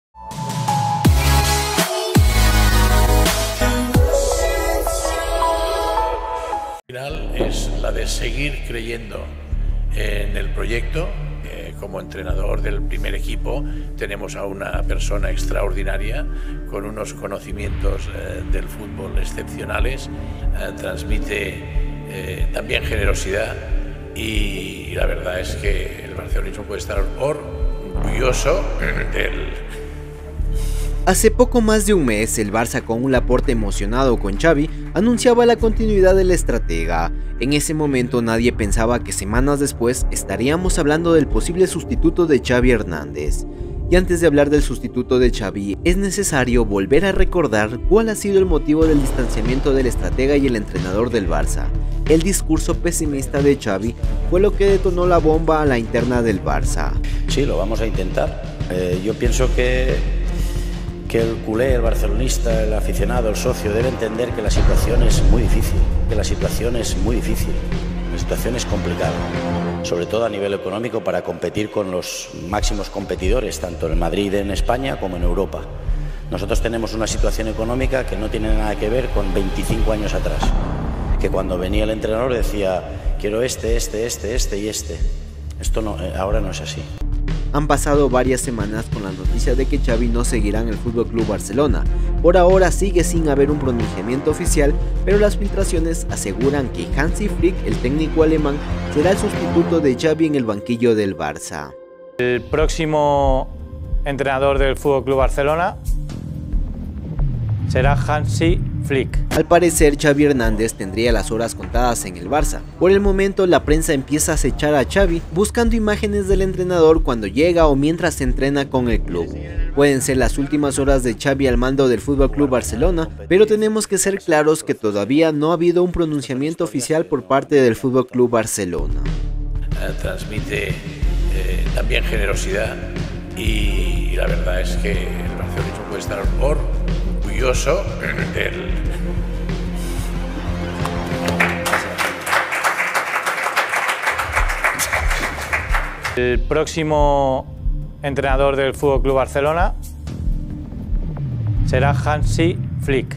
La REACCIÓN de XAVI al SABER que HANSI FLICK sería SU SUSTITUTO EN EL FC BARCELONA
Hace poco más de un mes, el FC Barcelona, con un Joan Laporta ilusionado por la continuidad de Xavi, anunciaba la renovación del estratega. Sin embargo, nadie imaginaba que semanas después estaríamos hablando del posible reemplazo de Xavi Hernandez.
Los rumores sobre la salida de Xavi se intensificaron tras un discurso pesimista del técnico donde expresaba su preocupación por la situación económica del club y la dificultad para competir con equipos como el Real Madrid.
Varias fuentes aseguran que Hansi Flick, ex entrenador del Bayern Munich, será el encargado de tomar las riendas del FC Barcelona la próxima temporada.
La decisión de la directiva ha generado opiniones divididas entre la afición culé. Algunos respaldan la decisión, considerando la experiencia y trayectoria de Flick, mientras que otros lamentan la salida de Xavi y su estilo de juego.

La salida de Xavi Hernandez marca el final de una corta pero intensa etapa en el banquillo del FC Barcelona. La llegada de Hansi Flick como su sustituto abre un nuevo capítulo en la historia del club, con un nuevo proyecto deportivo que busca devolver al Barça a la cima del fútbol español y europeo.